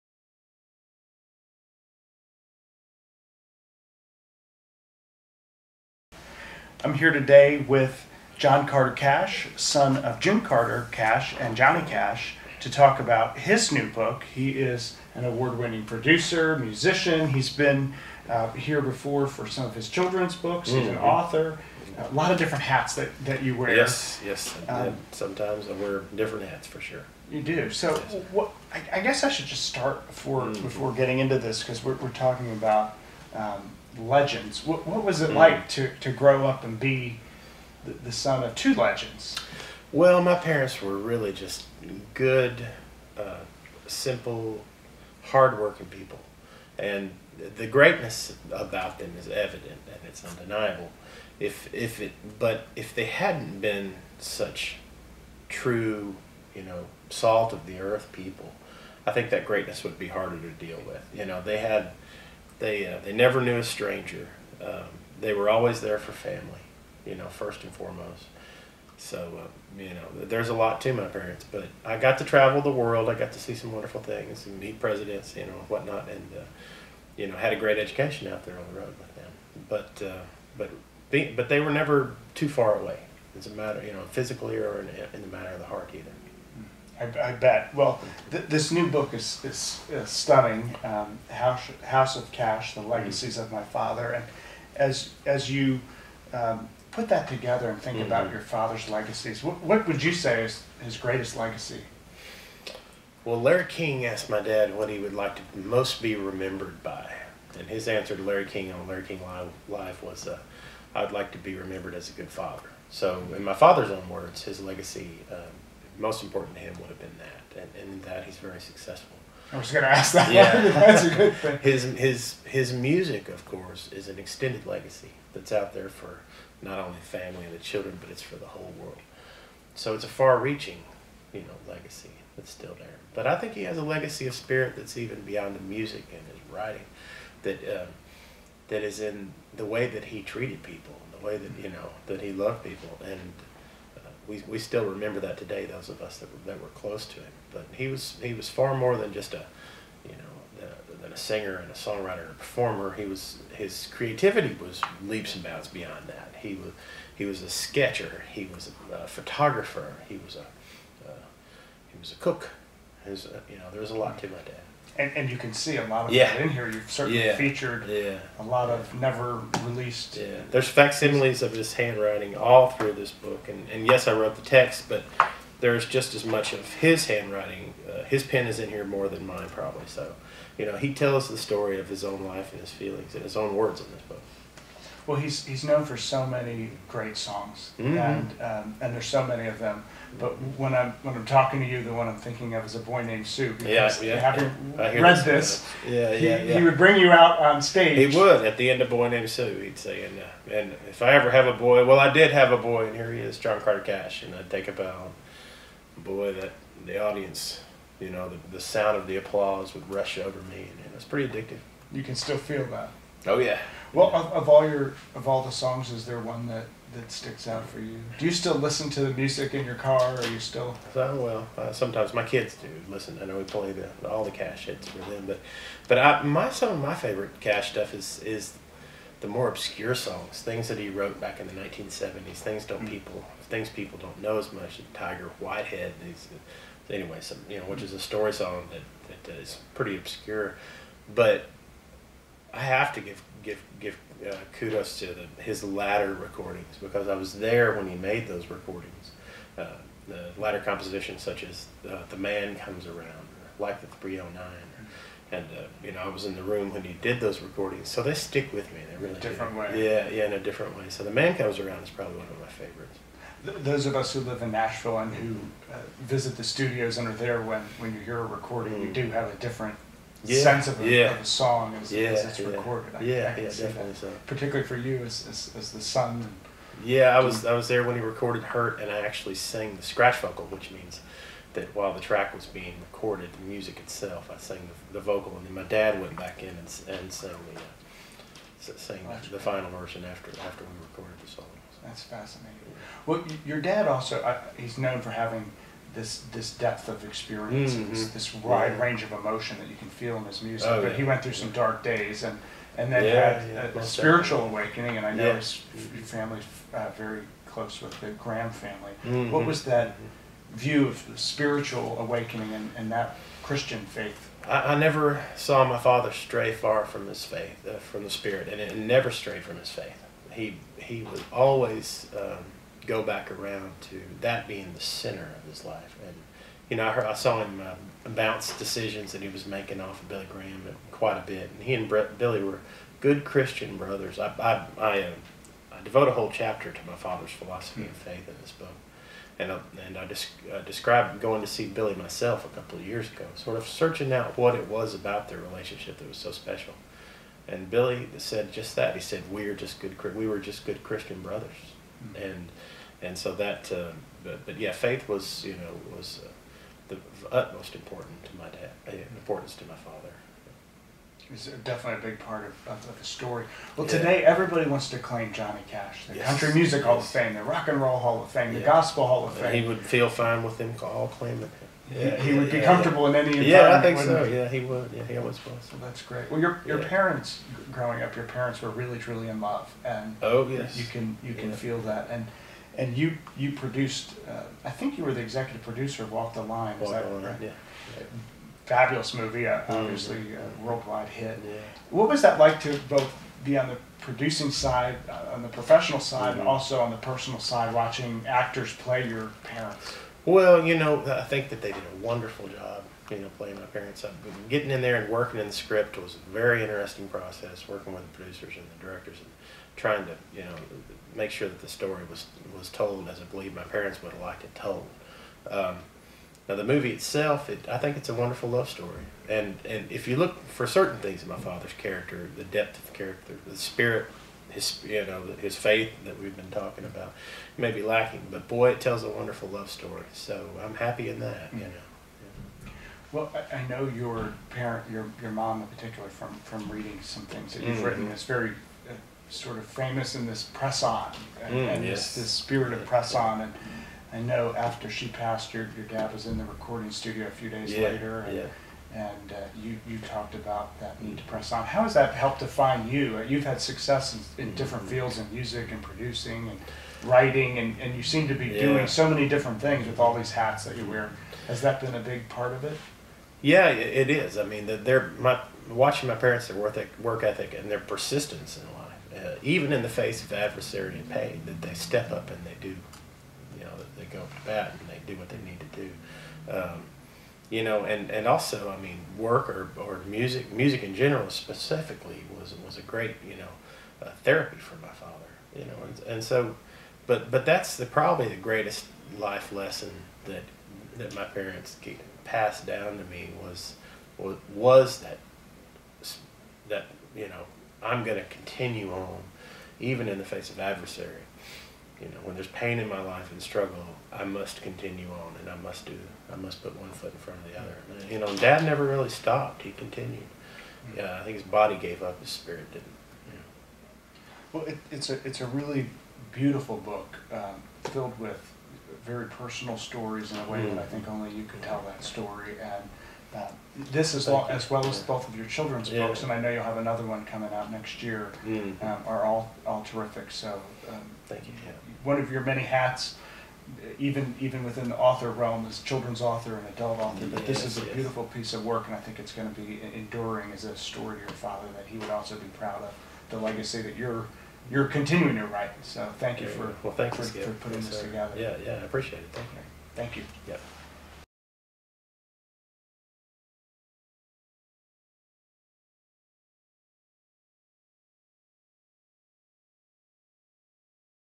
Carry on, with John Carter Cash
The Man In Black, Johnny Cash, and his wife June Carter Cash left a legacy of music that few can match. In this interview by Skip Prichard, their son, John Carter Cash, talks about Cash family life and his book, “House of Cash.”

Of his parents, John says, “They were two salt-of-the-earth people. They never met a stranger and they were always there for family first and foremost. I think he had a spirit that’s beyond music, beyond writing: the way that he treated people.”

John learned more than the music business from his folks. “Watching my parents’ work ethic and their persistence in life, even in the face of adversity and pain, they step up and they do what they need to do,” he says. “Probably the greatest life lesson that my parents passed down to me was I must continue on, I must put one foot in front of the other. Dad never really stopped,” says John. “His body gave up, but his spirit didn’t.”

Sending you the deepest appreciation for watching, learning, and connecting.

Wishing you a life full of meaning, happiness, and success!

Skip